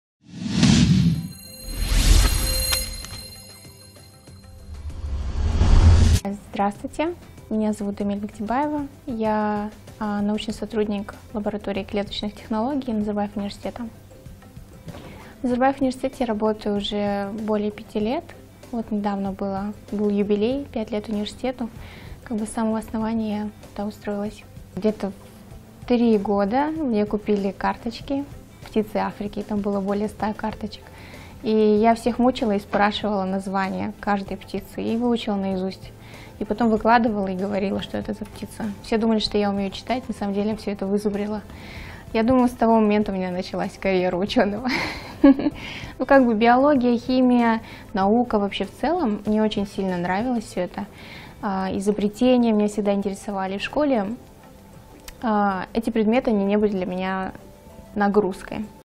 Новости инноваций проекты ONAI, Hoope
Казахстанский ученый-биотехнолог Дамель Мектепбаева вслед за диагностическим кольцом «Hoope», создала проект по внедрению электронной медицинской карты. Кроме этого, кольцо по выявлению венерических заболеваний вызвал огромный резонанс в отечественной медицине и не только. Спустя короткое время, Дамель Мектепбаева представила на всеобщее обозрение проект «Отпечаток  здоровья нации Onai».